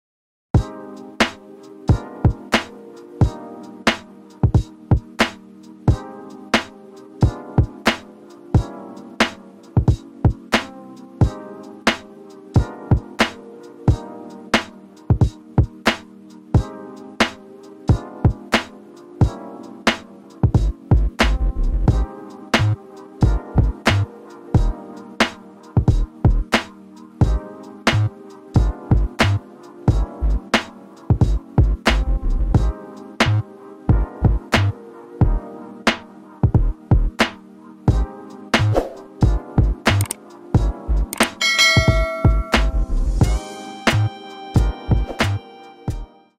adidas NMD R1 Also Returning in OG White
adidas NMD R1
Color: Cloud White/Cloud White-Cloud White
Style Code: GZ7922
Release Date: June 1, 2021
Price: $140